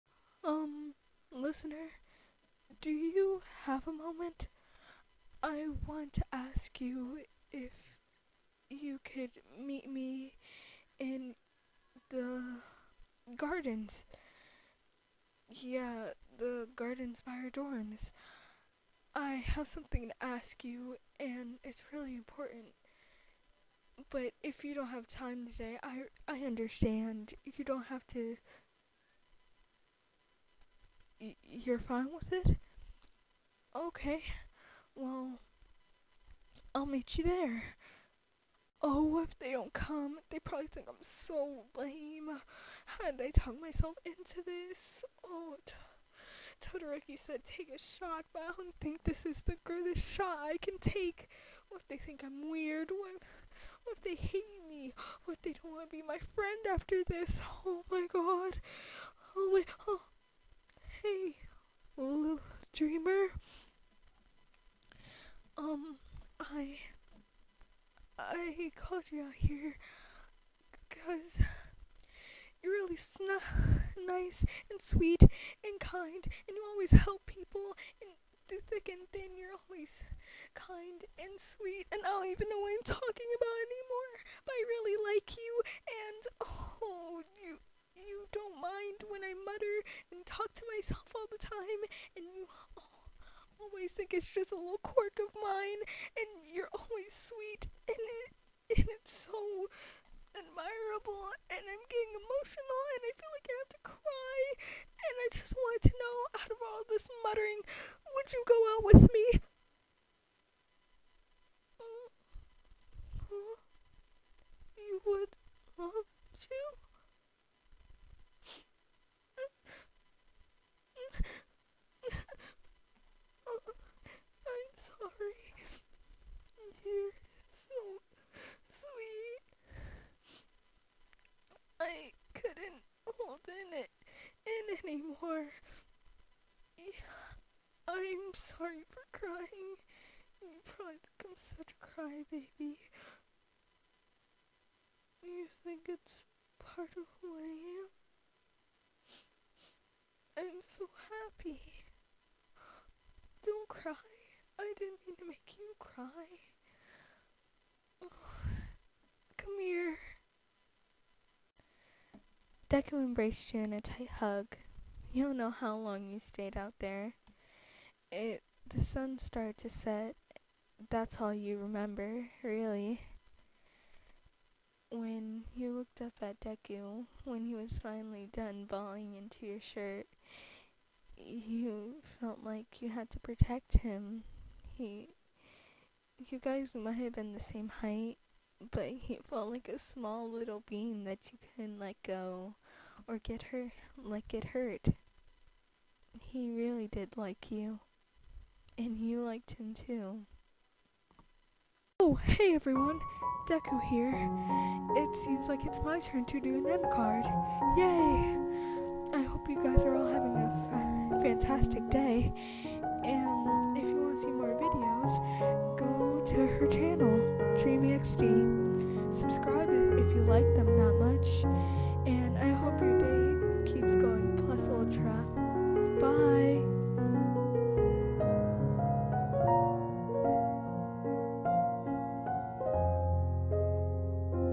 "You made me so red" { Deku x Listener }Confession
He mumbles that he like you...

💫Thank for supporting me Dreamers💫

P.S.💖✨ Keep Dreaming✨💖